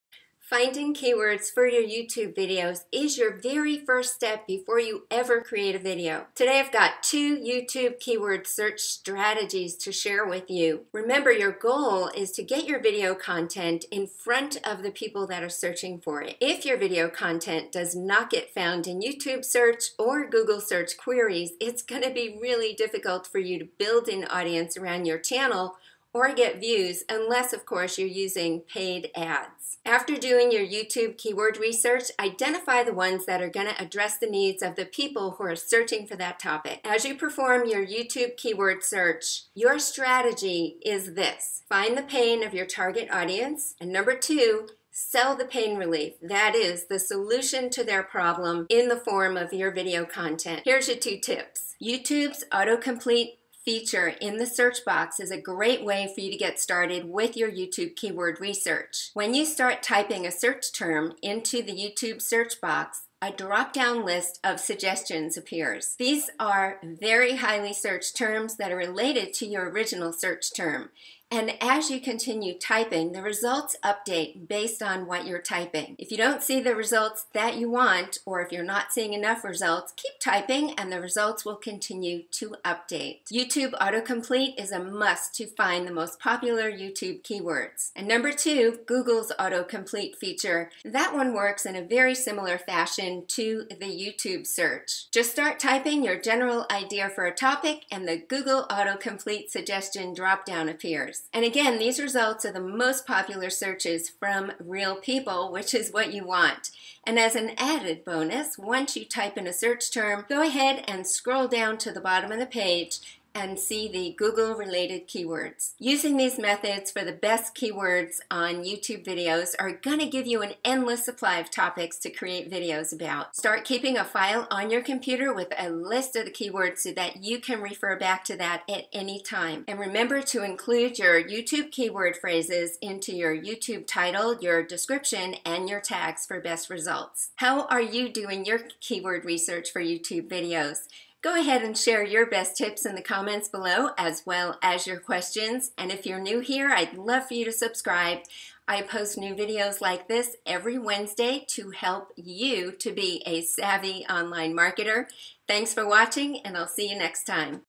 How to Find Keywords for YouTube Videos
Before creating videos, you need to find the best YouTube keywords. This helps get your video content in front of as many people as possible.

Get your videos found in YouTube search and Google search queries.
Finding the best keywords for YouTube videos will make it more possible for your channel to get discovered in search.

Be sure to do your YouTube video keyword research, identify the words and phrases that will address the needs of people who are searching for that topic.

As you perform your keyword research for videos, your strategy is to understand the type of information your audience is searching for and provide a solution to that problem.

Today I have two YouTube keyword search strategies to share:
YouTube Autocomplete Feature
Google  Autocomplete Feature

Using dong video keyword research you'll find the best phrases for YouTube videos will give an endless supply of topics to create videos about.

Be sure to include your YouTube keyword phrases into the video title, description and tags for best results. That is part of your SEO optimization.